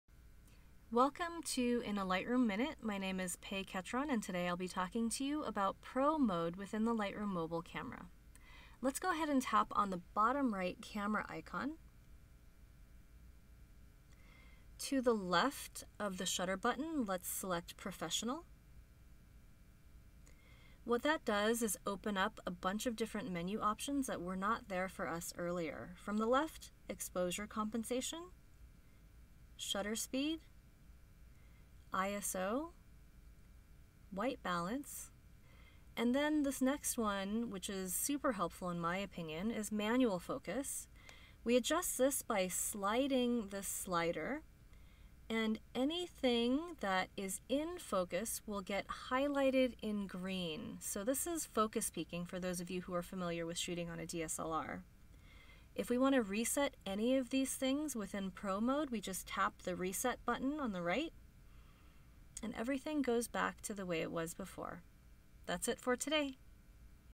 How to Access Pro Mode in Lightroom Mobile | In A Lightroom Minute | Adobe Lightroom
Welcome to InALightroomMinute! In this episode, Pei Ketron shares how to switch on Pro Mode within the Lightroom Mobile camera. Watch this video to discover how to make your photos a bit more professional using Lightroom Mobile.